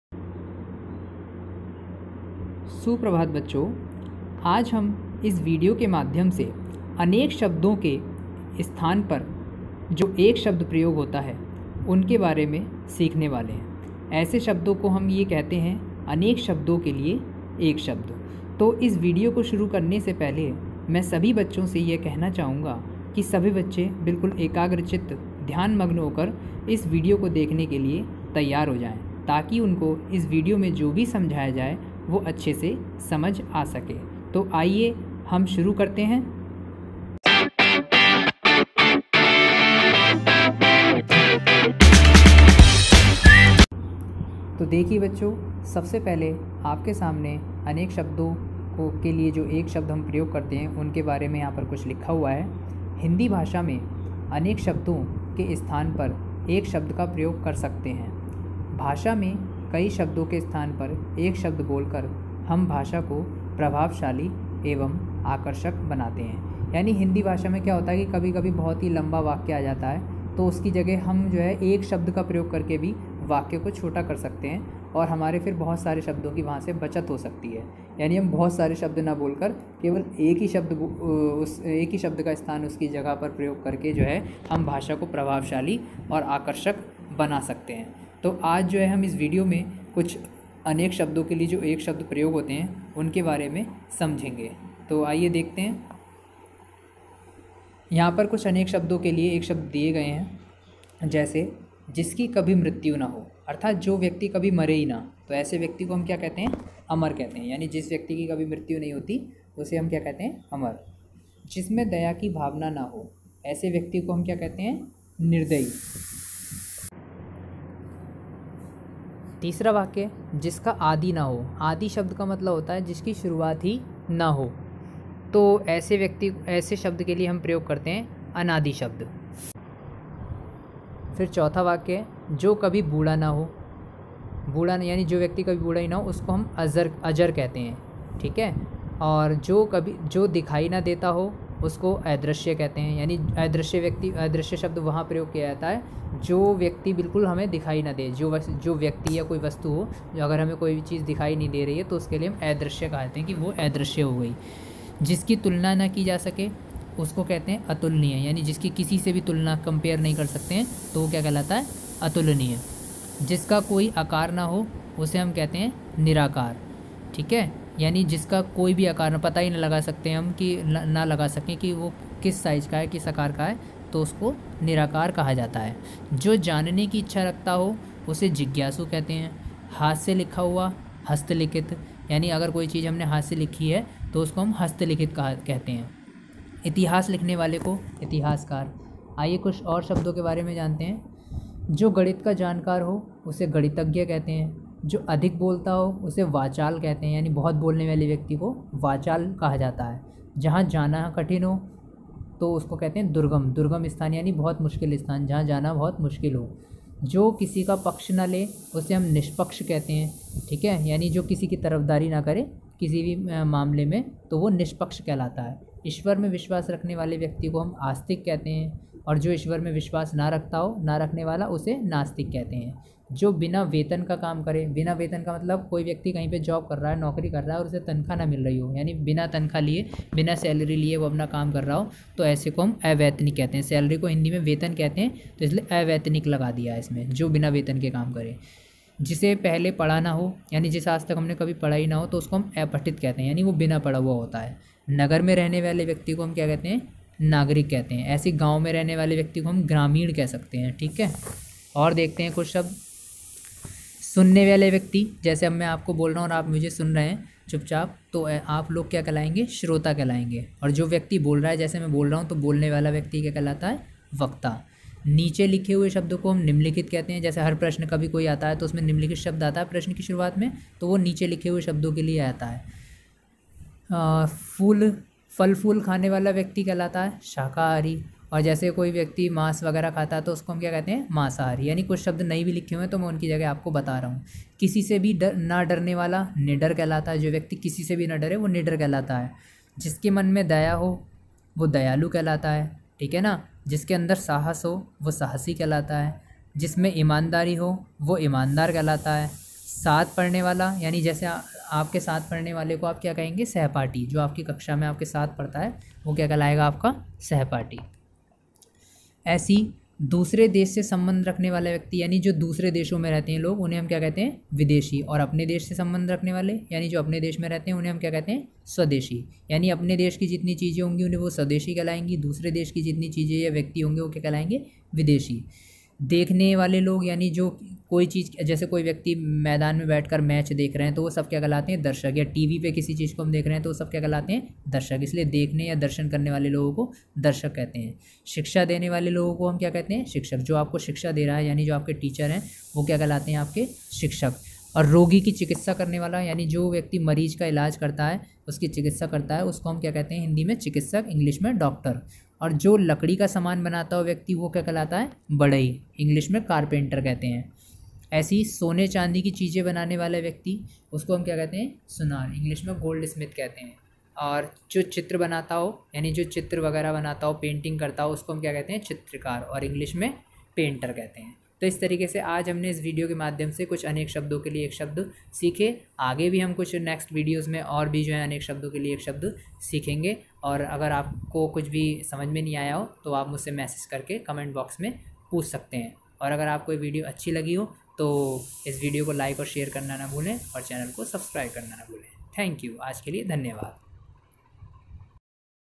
one word substitution in hindi ( अनेक शब्दों के लिए एक शब्द)
इस विडियो में हिंदी व्याकरण के अनेक शब्दों के लिए प्रयोग होने वाले एक शब्द के बारे में बताया गया है।